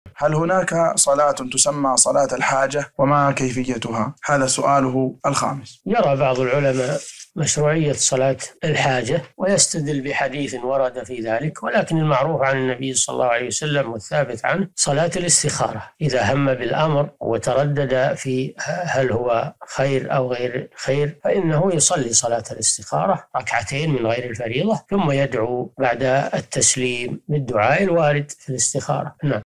5185- هل تشرع صلاة الحاجة؟ وما صفة صلاة الاستخارة؟ - الشيخ صالح الفوزان
"5185- هل هناك صلاة تسمى صلاة الحاجة؟ وما كيفيتها؟ هذا سؤاله الخامس.
الجواب: يرى بعض العلماء مشروعية صلاة الحاجة ويستدل بحديث ورد في ذلك ولكن المعروف عن النبي ﷺ والثابت عنه صلاة الاستخارة إذا همَّ بالأمر وتردد فيه هل هو خير أو غير خير فإنه يصلي صلاة الاستخارة ركعتين من غير الفريضة ثم يدعو بعد التسليم بالدعاء الوارد في الاستخارة.
السيرة الذاتية :
للاشتراك في قناة الشيخ صالح الفوزان

14_ الشيخ محمد أمين الشنقيطي
يتيوب

15_سماحة المفتي عبد العزيز آل الشيخ
 يوتيوب

رئيس الفريق
لمشروع كبار العلماء